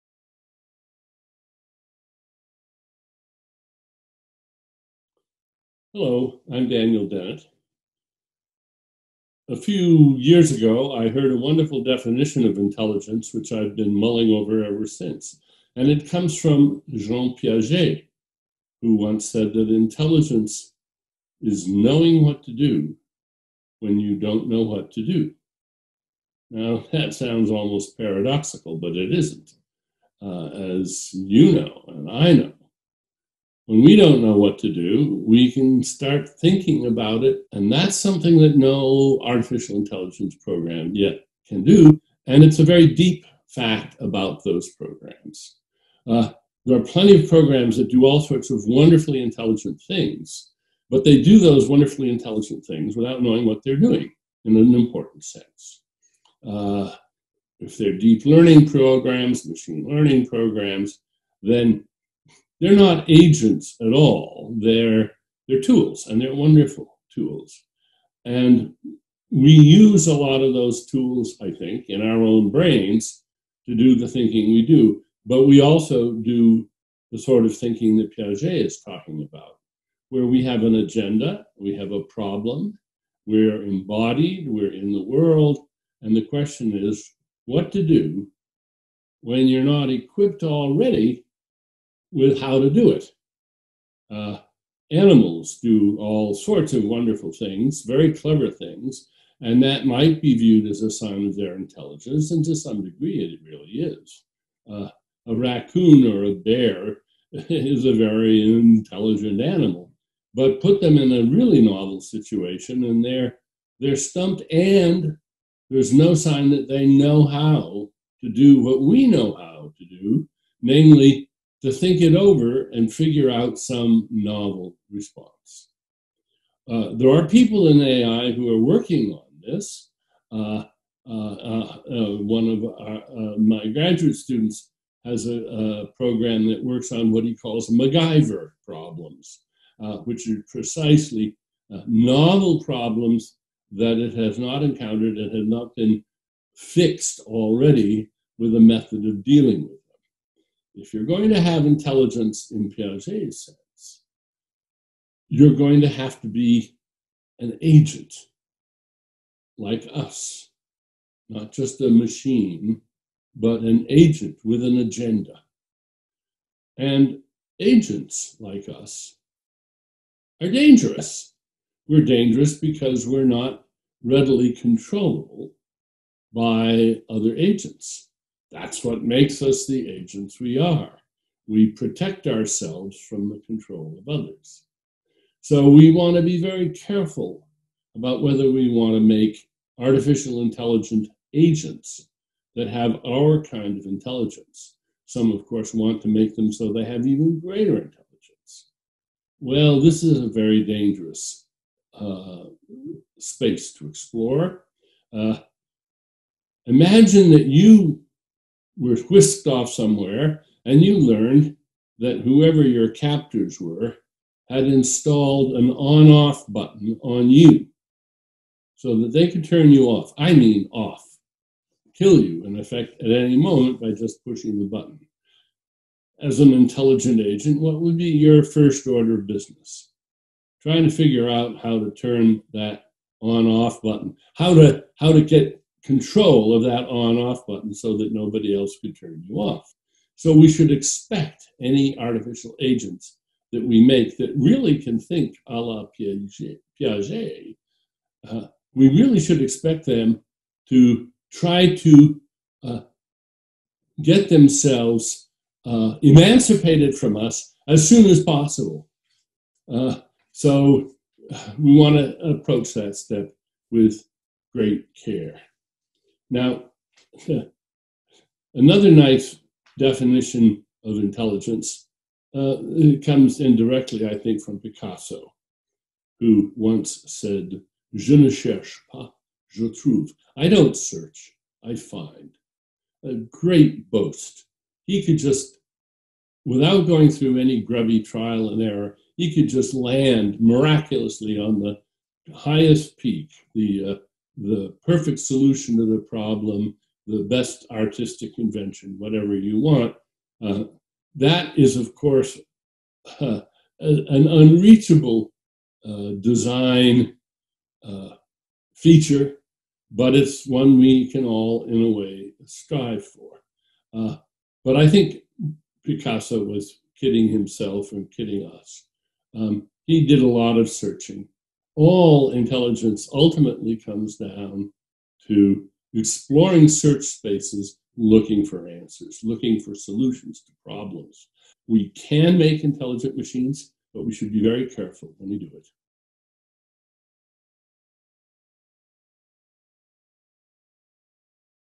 Daniel Dennett: The risks of creating artificially intelligent agents
Daniel Dennett speaking at CIFAR presents The Walrus Talks at Home: Intelligence. Daniel Dennett is an advisor for the CIFAR Brain, Mind & Consciousness program, a philosopher, and co-director of the Center for Cognitive Studies at Tufts University.